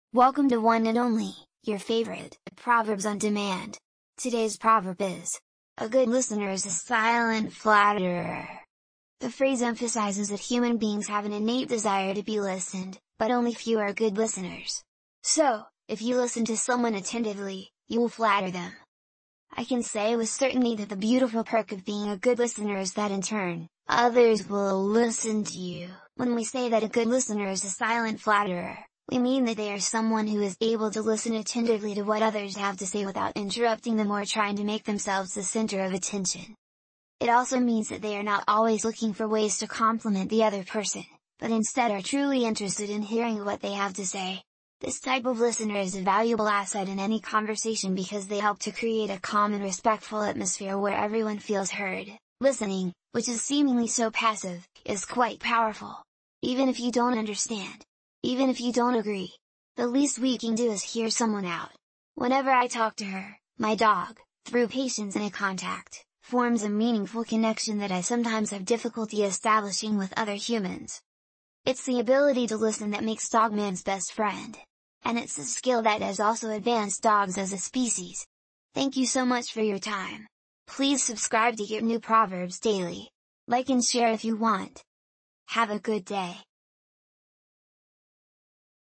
A Good Listener is a Silent Flatterer | Proverb | Meaning | Origin | Proverbs Explained | POD
Dive into the wisdom of active listening with "A Good Listener is a Silent Flatterer" in this episode of "Proverbs Explained." Explore the nuances of sincere attention and its impact on communication, relationships, and personal growth.

🌟 Don't miss out on the rich wisdom embedded in this proverb—watch the full explanation now!